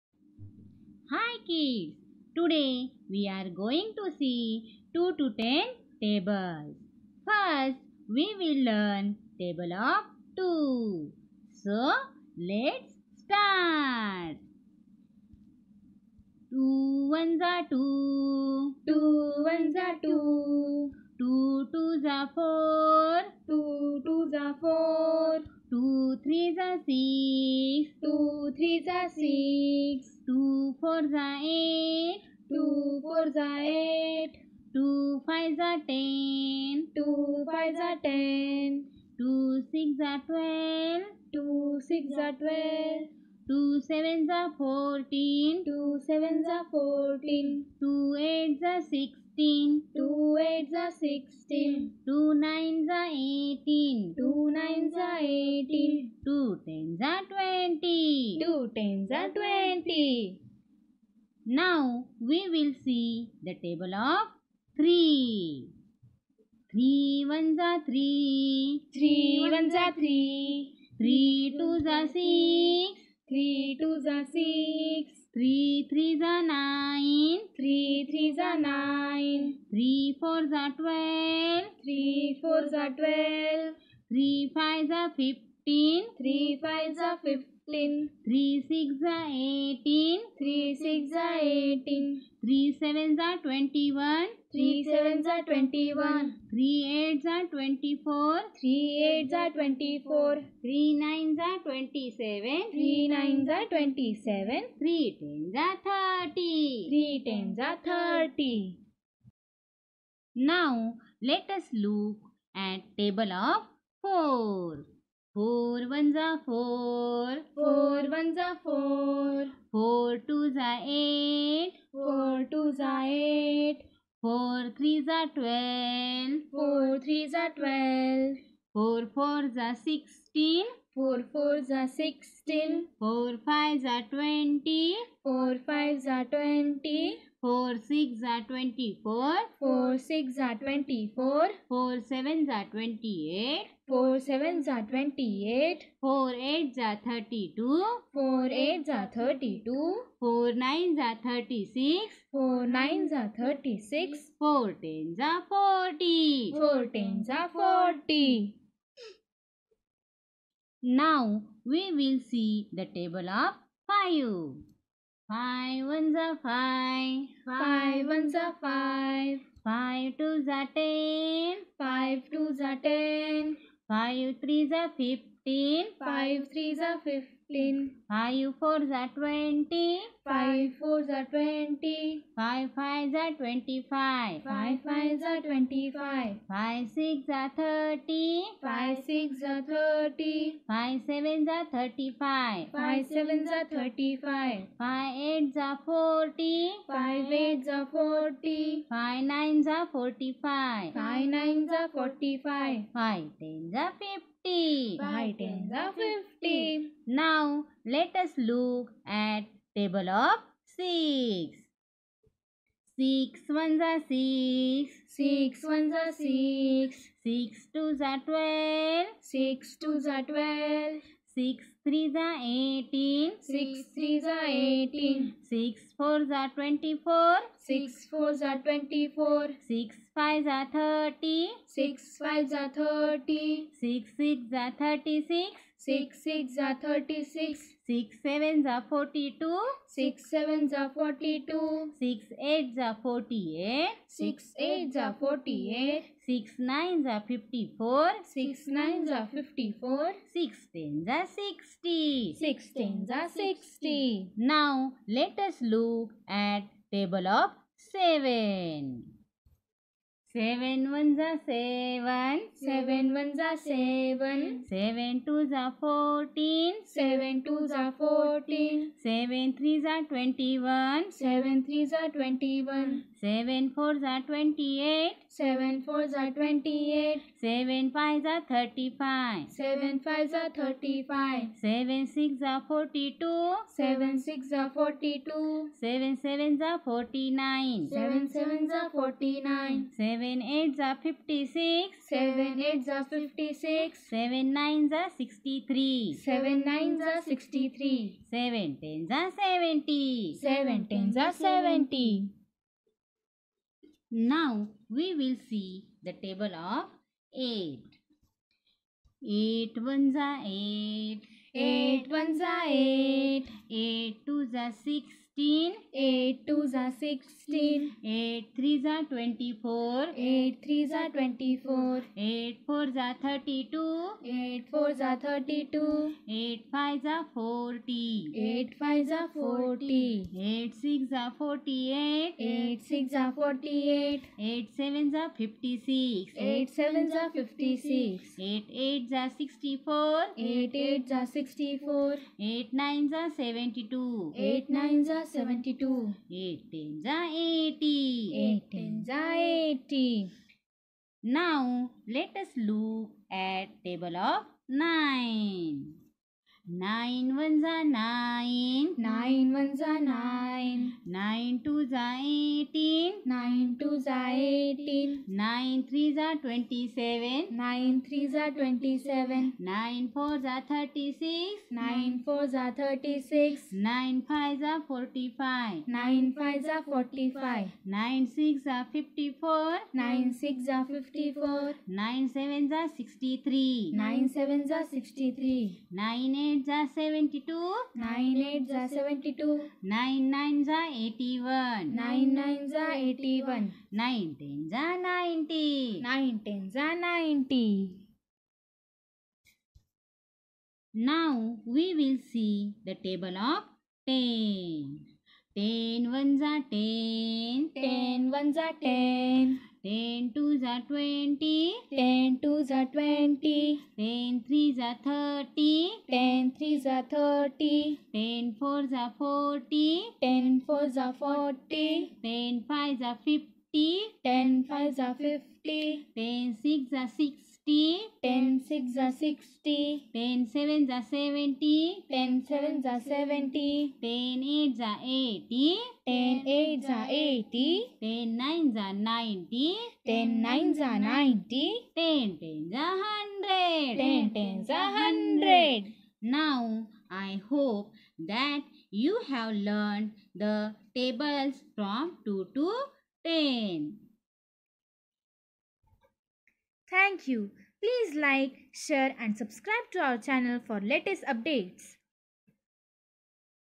2 - 10 tables | 2 to 10 table | Learn with ease | Arati's Education
Helloooo ! Kids !

Today we are here with another video for you ...
The video is of 2 - 10 tables for you.
We have also given you the very nice recitation of the tables in a very intresting way.

Please explore our another videos also ...

Stay healthy
Stay home
Stay safe